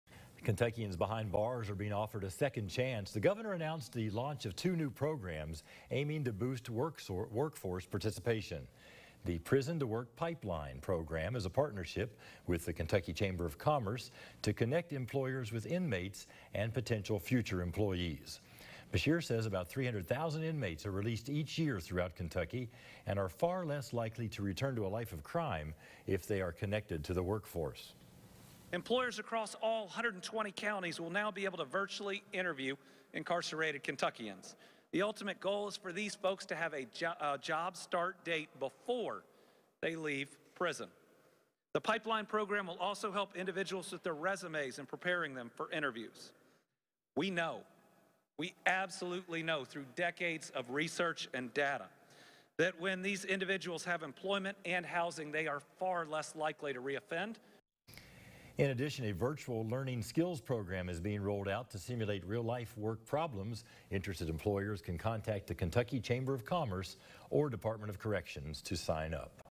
Kentucky governor promotes 'prison-to-work' program
Gov. Andy Beshear on Monday promoted a “prison-to-work” initiative aimed at offering second chances for Kentucky's inmates by having jobs lined up for them before they leave custody. The goal is to match inmates with Kentucky businesses in need of workers by allowing employers to virtually interview prisoners, the governor said at a news conference. Inmates also will receive help in writing resumes and preparing for interviews with prospective employers.